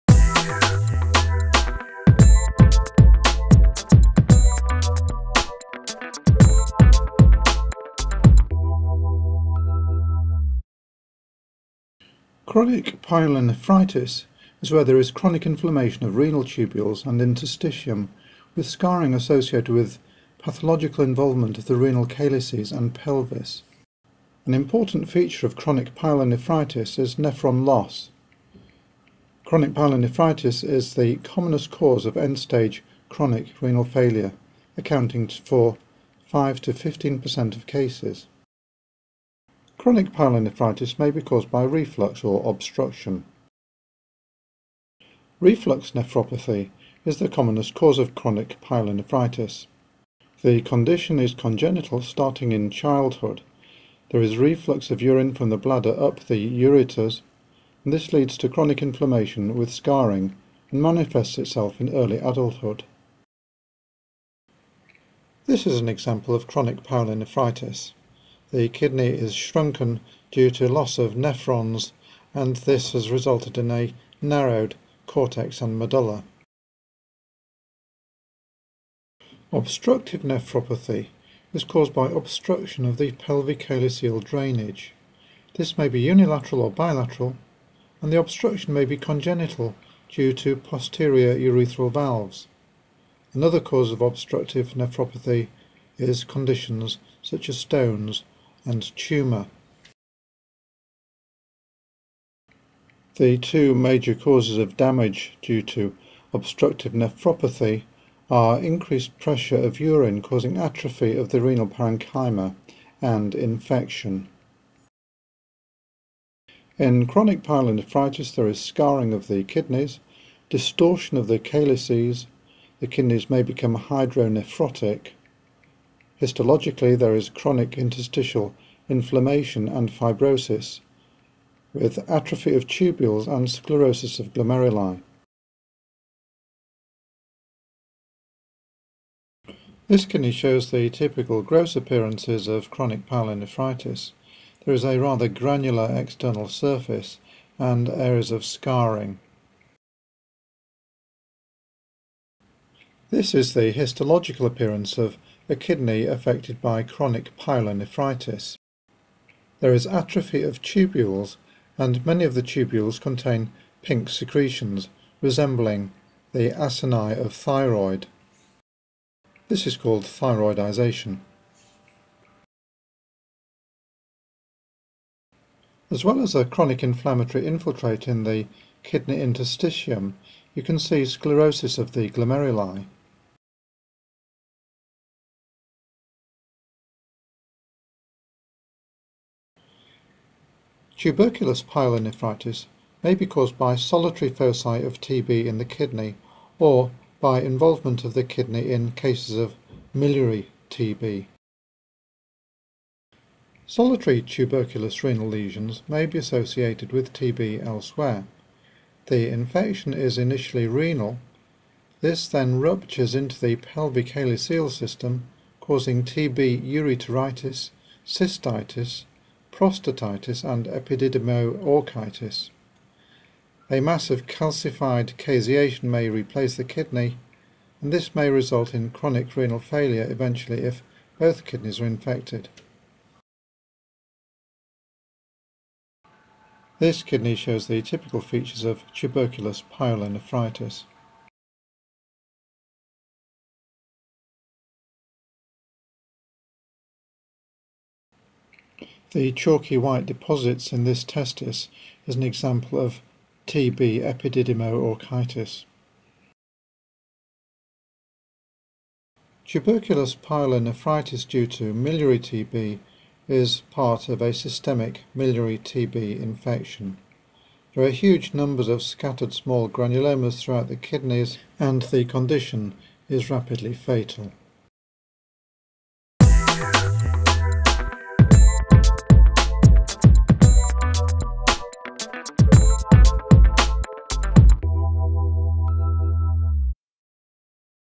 Chronic Pyelonephritis - Pathology mini tutorial
The aetiology, presentation, complications and morphology of chronic pyelonephritis
What is chronic pyelonephritis? 0:13
Causes of chronic pyelonephritis 0:39
Reflux nephropathy 0:47
Obstructive nephropathy 1:21
Morphology of chronic pyelonephritis 1:57
Gross appearance of chronic pyelonephritis 2:18
Histology of chronic pyelonephritis 2:33
TB pyelonephritis 3:13
Gross appearance of TB pyelonephritis 3:57